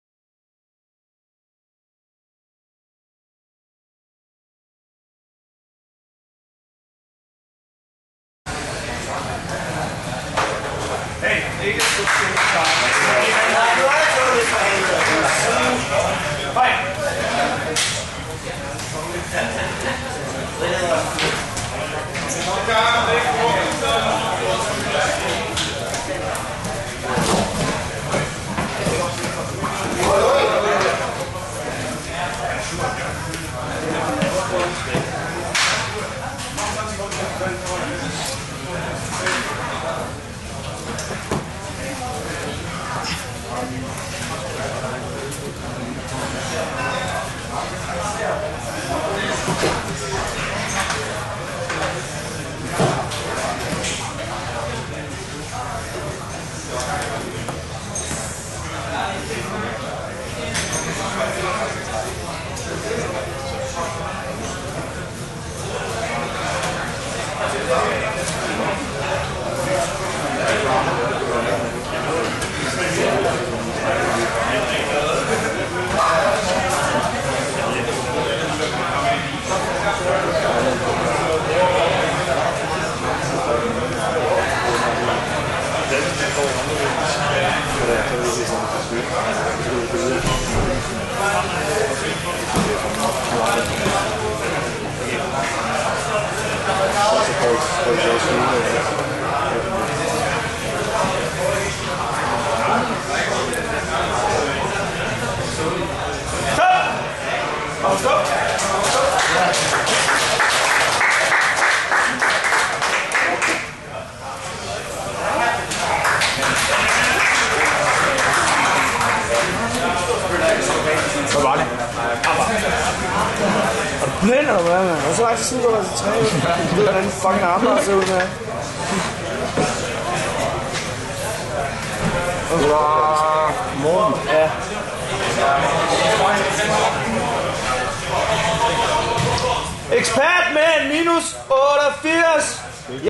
DGL DGL September 2017 Marcin Samson CSA vs Morten Hastoft CSA.dk
Dansk Grappling Liga 2017 DGL September Sjælland

Gruppe A  Øved Mænd -77.0 kg 5 min r.